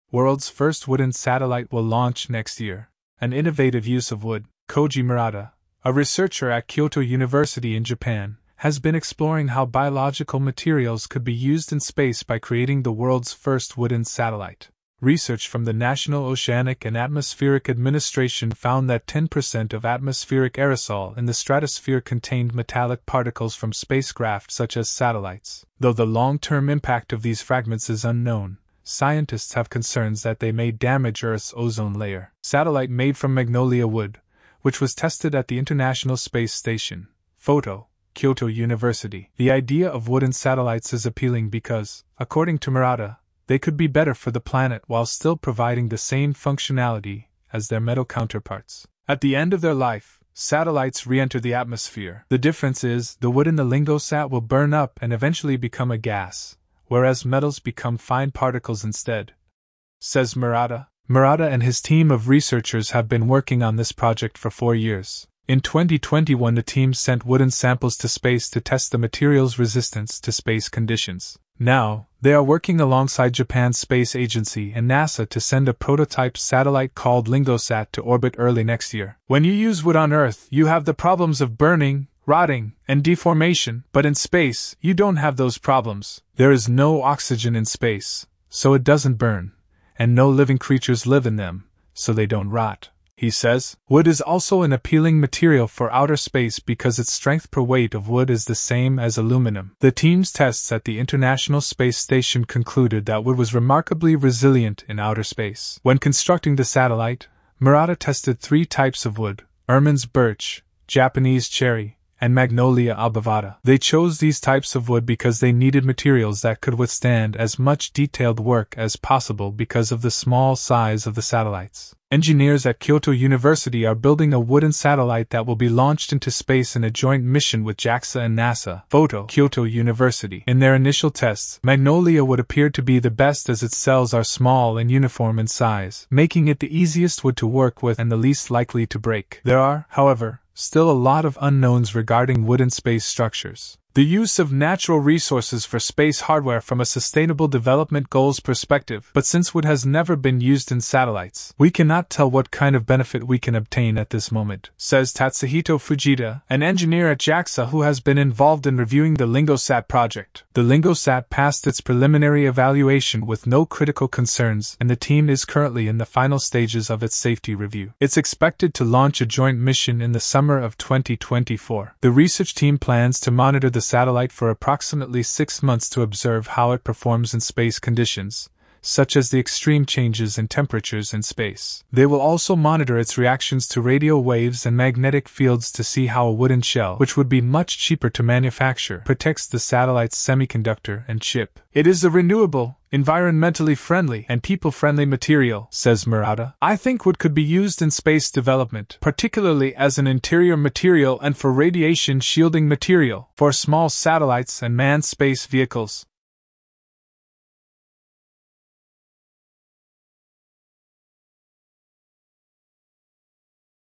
The World's first wooden satellite Complete details
Explore the groundbreaking achievement of the world's first wooden satellite in this captivating video. Learn about the innovative technology, environmental considerations, and the team behind this sustainable leap in space exploration. Discover how wooden materials are challenging traditional satellite construction and paving the way for eco-friendly solutions in outer space.